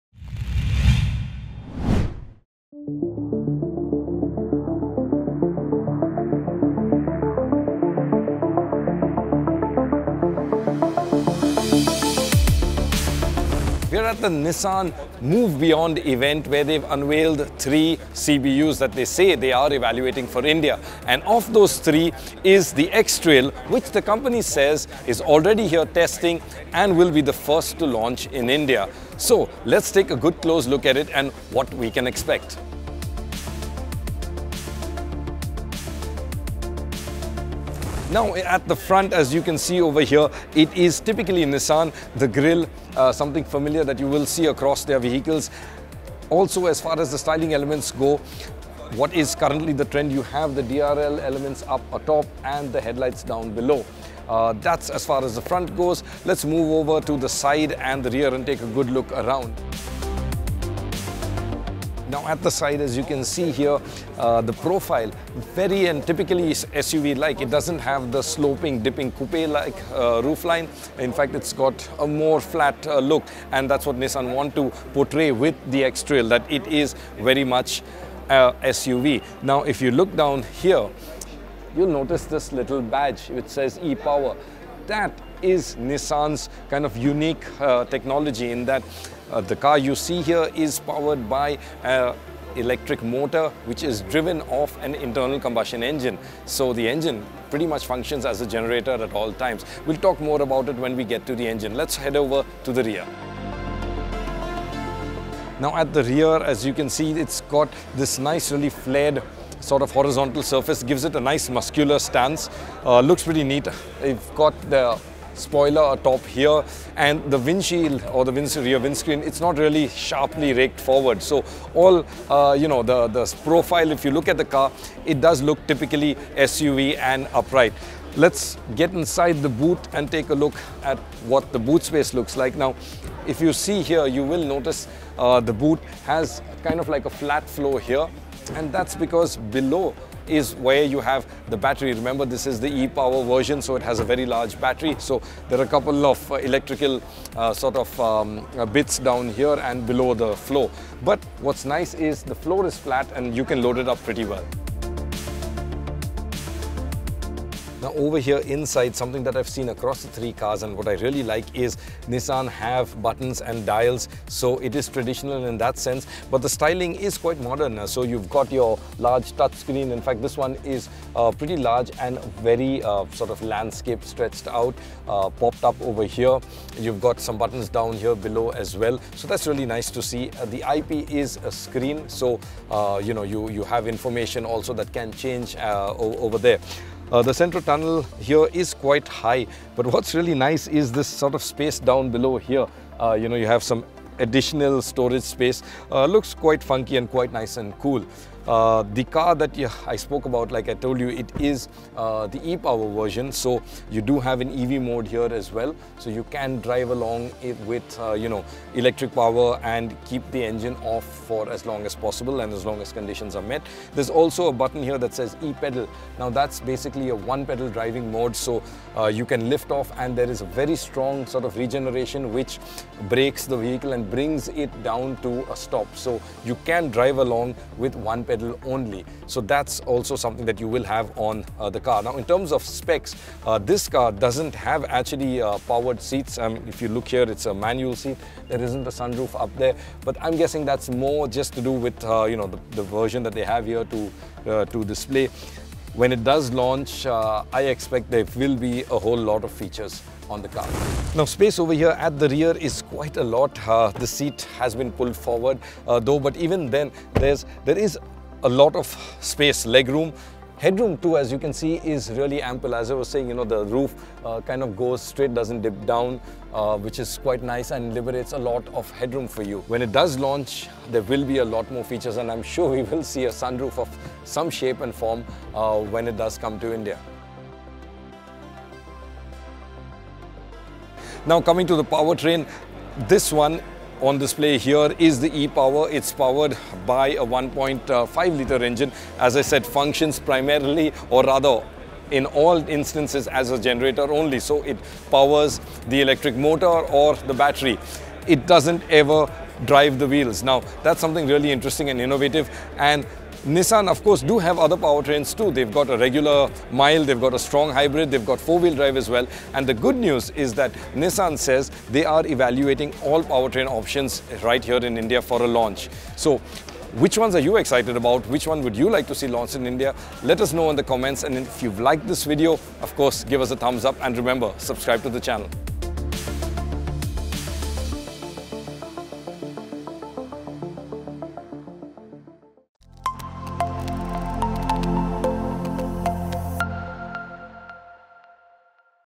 New Nissan X-Trail - Will it come back into the SUV fight? | Walkaround | Autocar India
Nissan unveiled the new X-Trail in India, but still hasn't confirmed its official launch. However, chances of it making to our shores are high and Sergius Barretto takes a quick tour on what to expect if and when it does.

Cameraperson: Anand Malepu
Editor: Pankaj Singh

0:00 - Intro
0:38 - Exterior design
2:12 - Boot capacity
2:35 - Interior
4:11 - Rear seat space
4:39 - Powertrain and engine